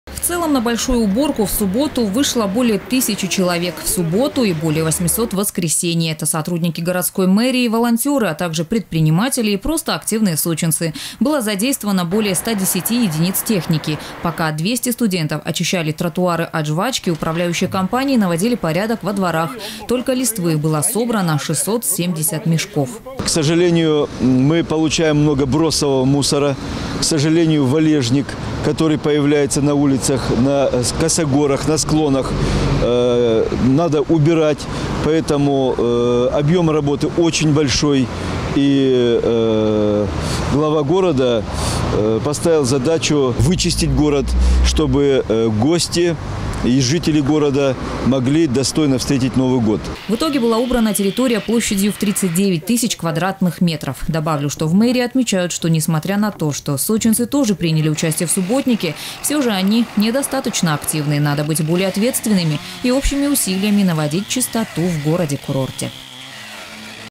Почти две тысячи человек приняли участие в общегородском субботнике в Сочи. Новости Эфкате
24 и 25 декабря в Сочи прошёл общегородской субботник. Проводить такую большую уборку, на курорте стало уже традицией. На этот раз поводом стали предстоящие новогодние праздники.

В целом на большую уборку в субботу вышло более тысячи человек в субботу и более 800 в воскресенье. Это сотрудники городской мэрии и волонтёры, а также предприниматели и просто активные сочинцы. Было задействовано более 110 единиц техники. Пока 200 студентов очищали тротуары от жвачки, управляющие компании наводили порядок во дворах. Только листвы было собрано 670 мешков.

Сочи. Новости.
Телеканал Эфкате.